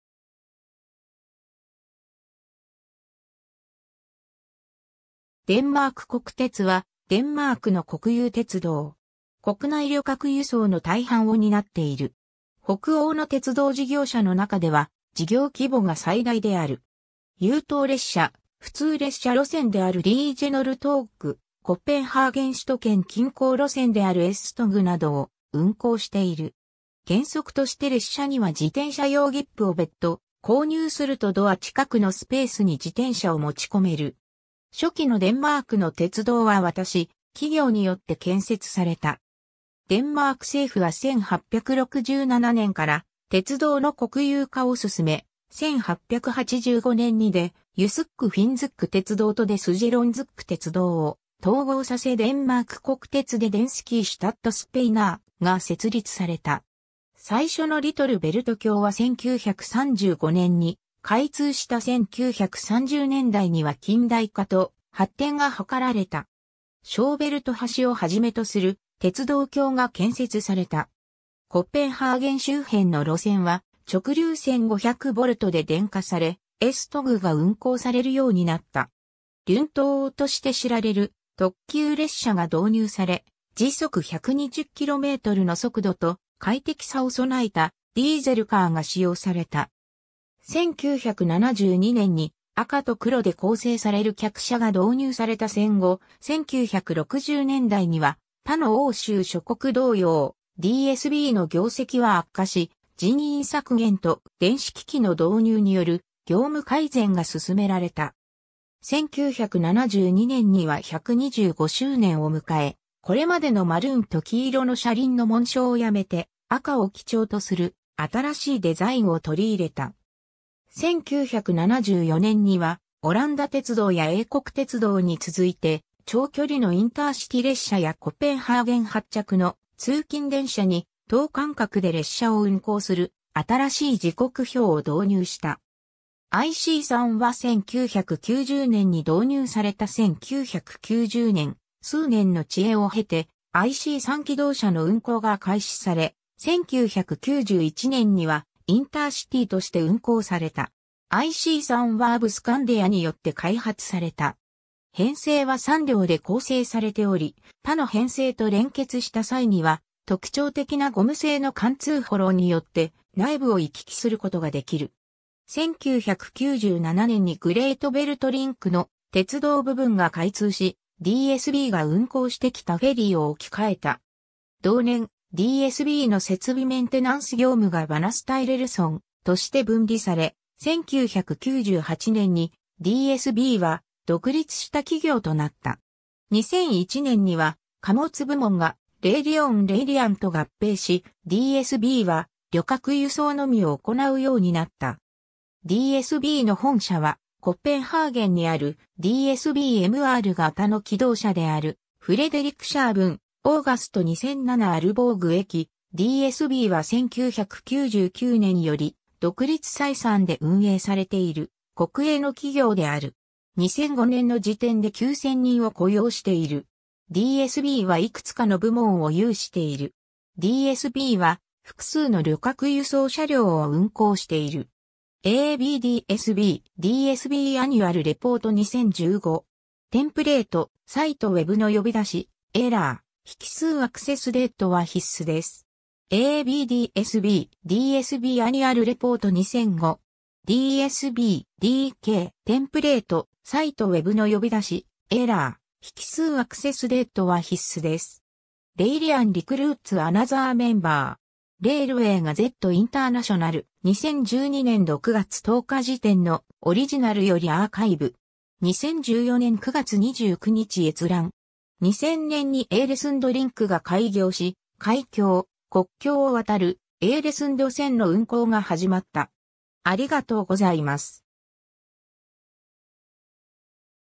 デンマーク国鉄
デンマーク国鉄（Danske Statsbaner (発音 [ˈtænskə ˈstɛˀtsˌpɛːnɐ]), Danish State Railways、以下DSB）はデンマークの国有鉄道。
国内旅客輸送の大半を担っている。
北欧の鉄道事業者の中では事業規模が最大である。
 優等列車、普通列車路線である Regionaltog (Regional trains) 、コペンハーゲン首都圏近郊路線であるS-Togなどを運行している。
 原則として列車には自転車用切符を別途購入するとドア近くのスペースに自転車を持ち込める。
 初期のデンマークの鉄道は私企業によって建設された。
デンマーク政府は1867年から鉄道の国有化を進め、1885年にDe jysk-fynske鉄道とDe sjællandske鉄道を統合させデンマーク国鉄De danske Statsbaner (DSB)が設立された。
 最初のLittle Belt橋は1935年に開通した 1930年代には近代化と発展が図られた。
小ベルト橋をはじめとする鉄道橋が建設された。
コペンハーゲン周辺の路線は直流1500Vで電化され、S-Togが運行されるようになった。
リュントー(lyntog) として知られる特急列車が導入され、時速120kmの速度と快適さを備えたディーゼルカーが使用された。
 1972年に赤と黒で構成される客車が導入された 戦後、1960年代には他の欧州諸国同様DSBの業績は悪化し、人員削減と電子機器の導入による業務改善が進められた。
1972年には125周年を迎え、これまでのマルーンと黄色の車輪の紋章をやめて赤を基調とする新しいデザインを採りいれた。
1974年には、オランダ鉄道や英国鉄道に続いて、長距離のインターシティ列車やコペンハーゲン発着の通勤電車に、等間隔で列車を運行する新しい時刻表を導入した。
 IC3は1990年に導入された 1990年、数年の遅延を経てIC3気動車の運行が開始され、1991年にはインターシティーとして運行された。
IC3はABBスカンディア(後にアドトランツに吸収)によって開発された。
編成は3両で構成されており、他の編成と連結した際には特徴的なゴム製の貫通幌によって内部を行き来することができる。
 1997年にグレートベルト・リンクの鉄道部分が開通し、DSBが運行してきたフェリーを置き換えた。
同年、DSBの設備メンテナンス業務がBanestyrelsenとして分離され、1998年にDSBは独立した企業となった。
2001年には貨物部門がレイリオンRailion (現在のDB Schenker Rail)と合併し、DSBは旅客輸送のみを行うようになった。
 DSBの本社はコペンハーゲンにある DSB "MR" 型の気動車であるFrederikshavn, August 2007 Aalborg駅 DSBは1999年より独立採算で運営されている、国営の企業である。
2005年の時点で9000人を雇用している。
 DSBはいくつかの部門を有している。
 DSBは複数の旅客輸送車両を運行している。
 ^ a b DSB. “DSB annual report 2015”. Template:Cite webの呼び出しエラー：引数 accessdate は必須です。
 ^ a b DSB. “DSB annual report 2005”. dsb.dk. Template:Cite webの呼び出しエラー：引数 accessdate は必須です。
 ^ “Railion recruits another member”. Railway Gazette International (2001年1月1日). 2012年6月10日時点のオリジナルよりアーカイブ。
2014年9月29日閲覧。
 ^ 2000年にエーレスンド・リンクが開業し海峡・国境を渡るエーレスンド線の運行が始まった。